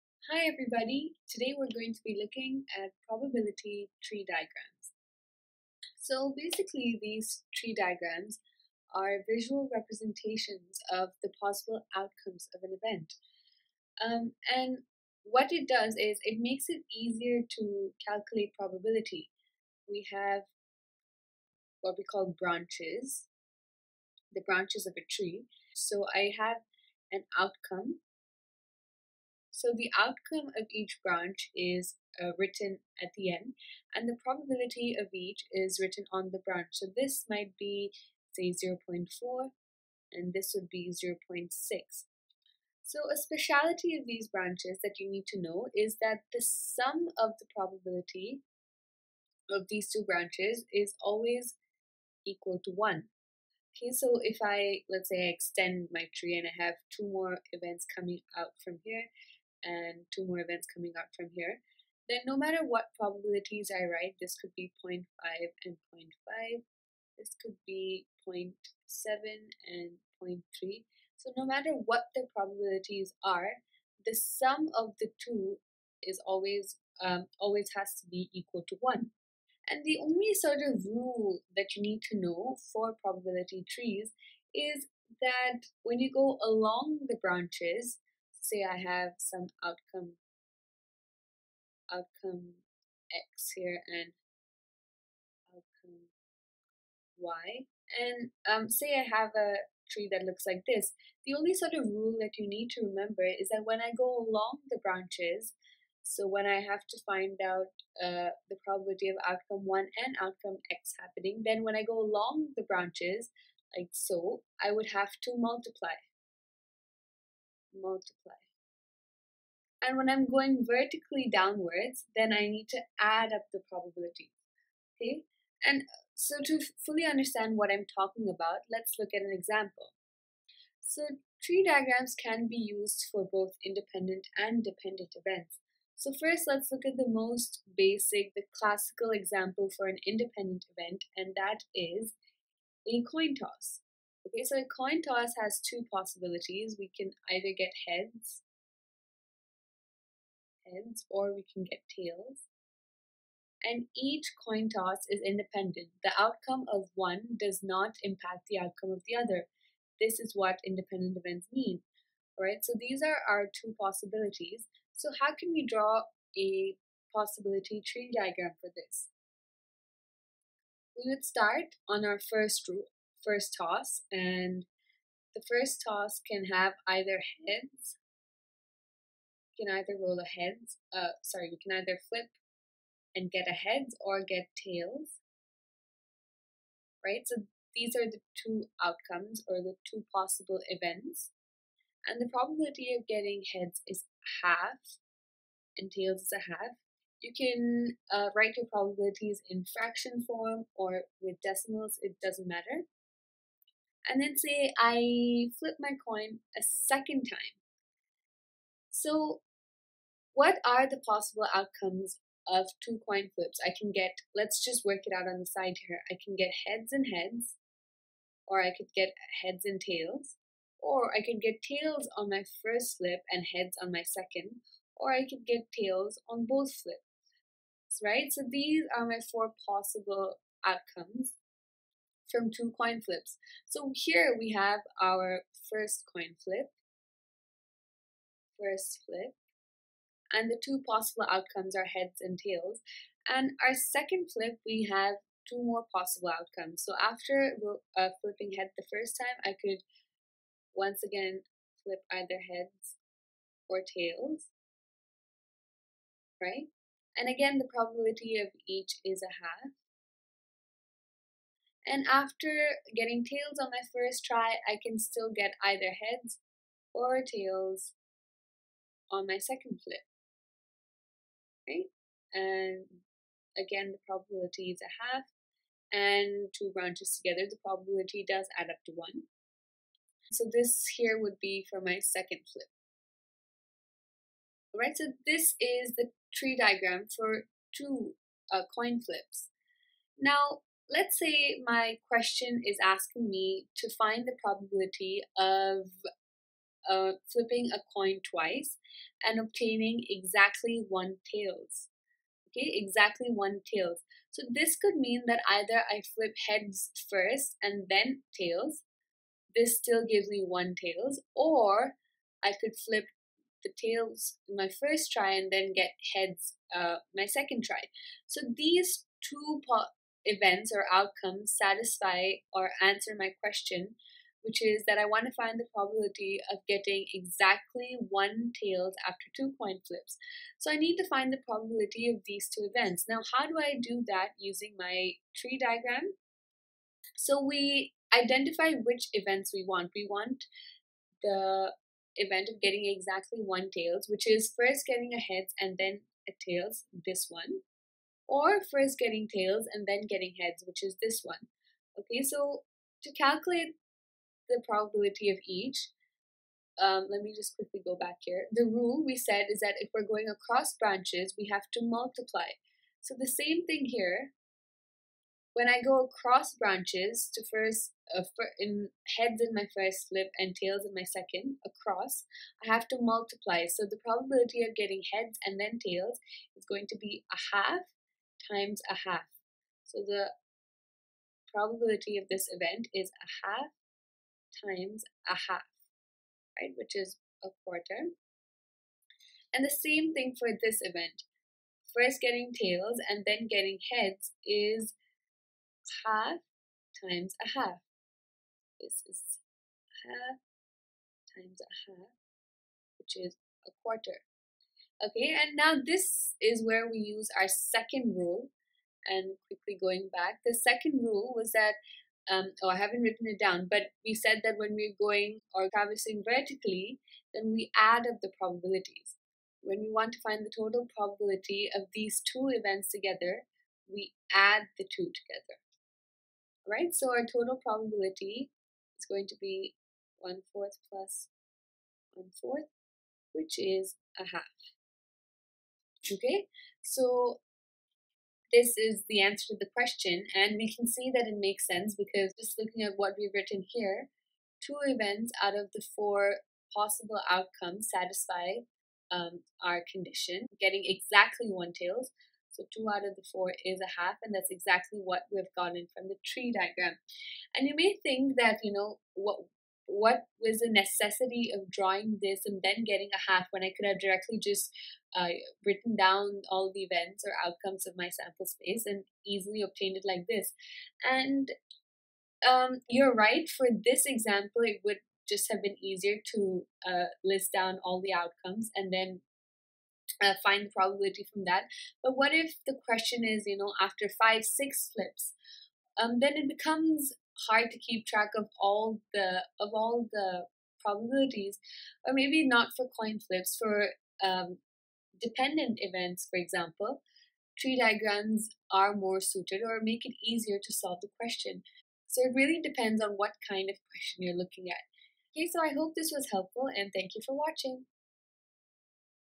Probability Tree Diagrams | Independent Events | With Example
This video provides a detailed explanation of probability tree diagrams, how to draw them, and the two main rules to follow while analyzing them.

Further, the video covers a classic example of independent events using tree diagrams - coin flips! In particular, the question being solved is "what is the probability of obtaining exactly one tail from two coin flips?".

The second part of this video is about dependent events using probability tree diagrams.